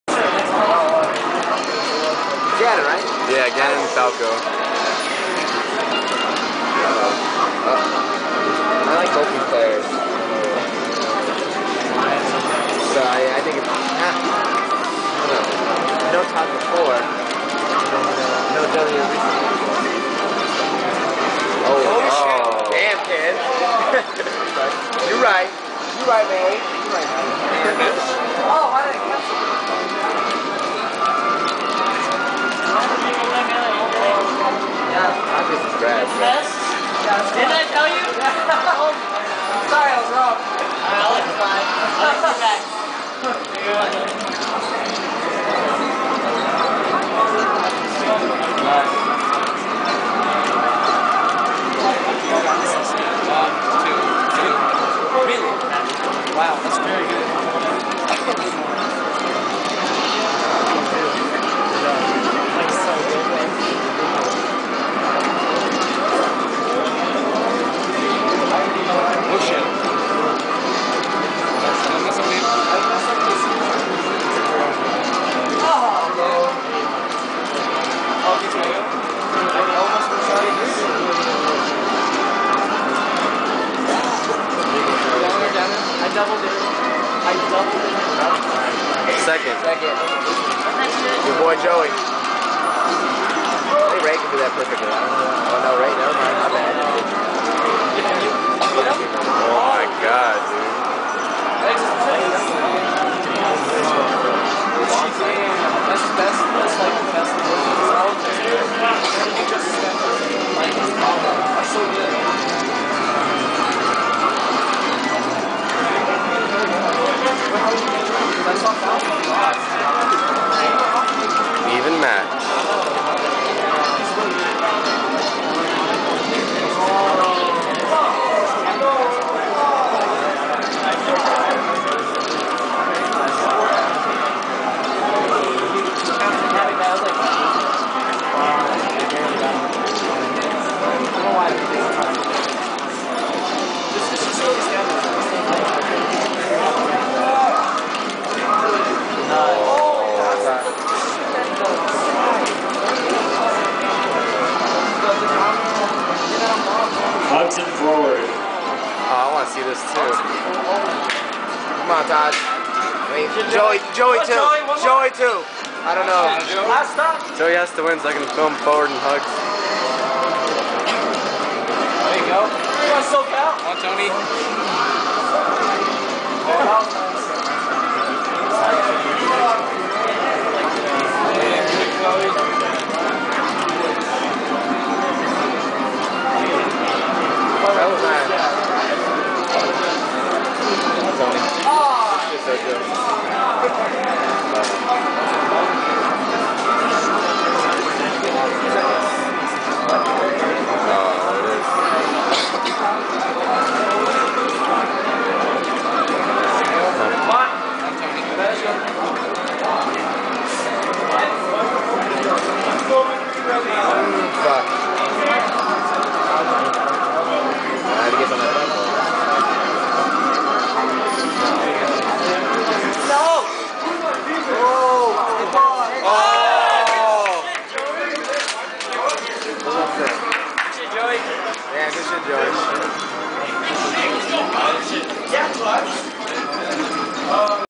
Losers Bracket Lucky(Fox) vs Taj(Marth) 2
Lucky vs Taj in singles losers bracket at Mango Juice